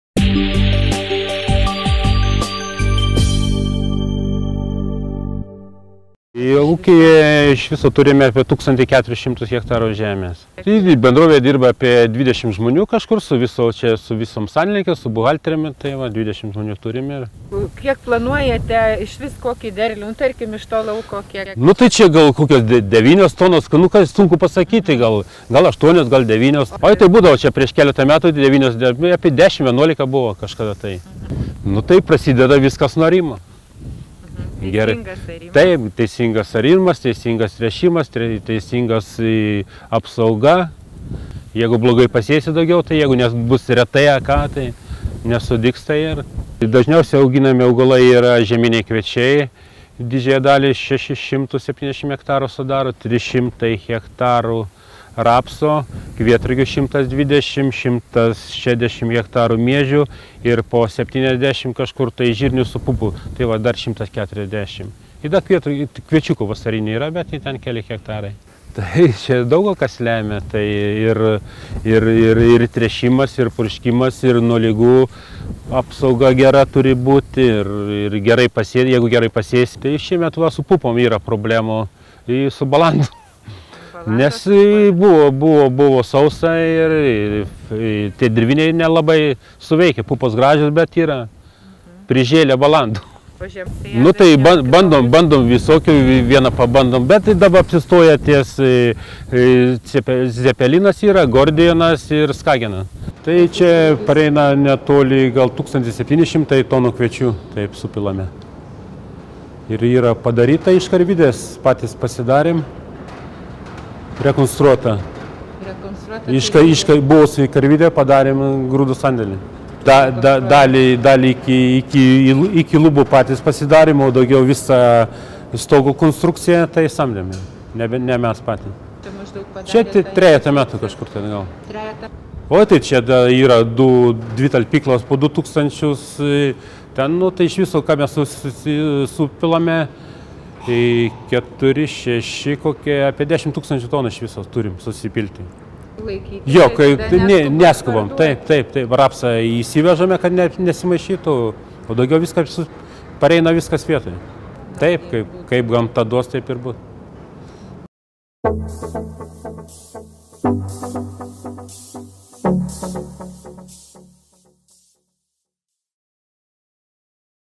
Javapjūtė prasidėjo. Tikimasi 8–9 t iš hektaro
Joniškio rajone įsikūrusios Alsių žemės ūkio bendrovės laukai driekiasi 1320 ha plote, kurio daugiau nei trečdalis – nuosava žemė. Pagrindinė bendrovės veiklos kryptis – augalininkystė.  Kasmet sėjami žieminiai kviečiai, rapsai, miežiai, kvietrugiai, žirniai ir pupos.
Bendrovėje jau pradėti šių metų derliaus nuėmimo darbai. Kūlami rapsai. Pasak Alsių žemės ūkio bendrovės vadovo, agronomo Romučio Lipšto, pernai tokiu metu darbas jau virte vire. Ši vasara nėra tokia dosni.

Red. Jolanta Abarienė, oper. Egidijus Vilkevičius